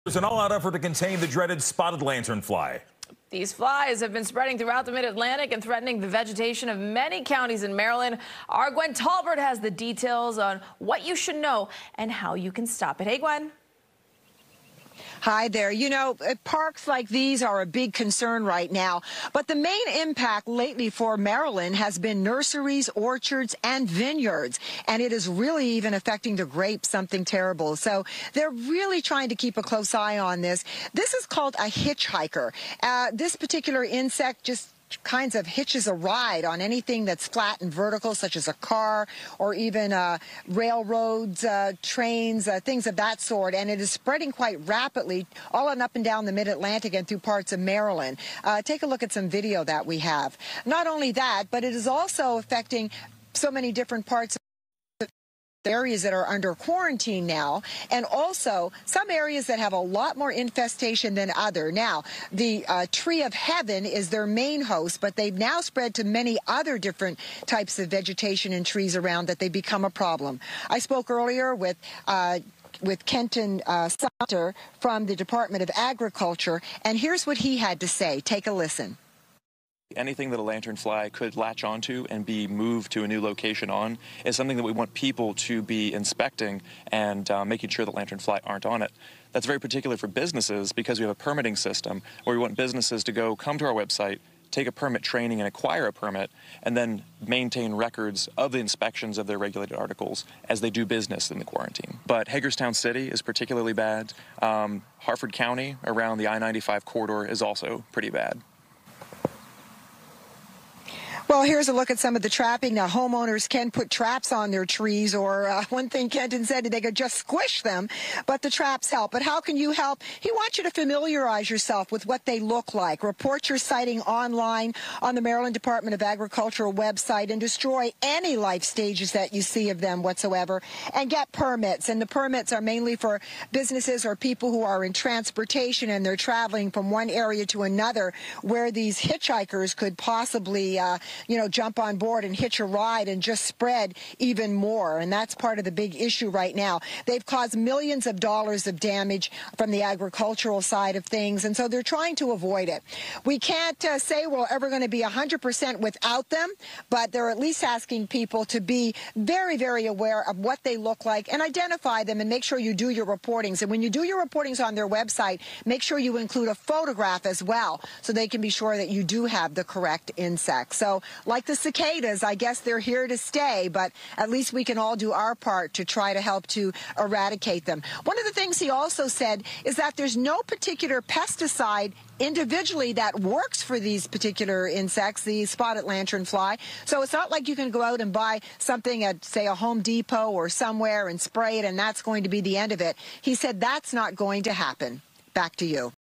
Beware of spotted lanternflies, Maryland officials warn | FOX 5 DC
The Maryland National Capital Park and Planning Commission is helping get the word out on the destructive Spotted Lantern Fly, which has been spreading throughout the Mid-Atlantic and threatening the vegetation of many counties in Maryland. FOX 5's Gwen Tolbart has the details on what you need to know and how to help stop it. MORE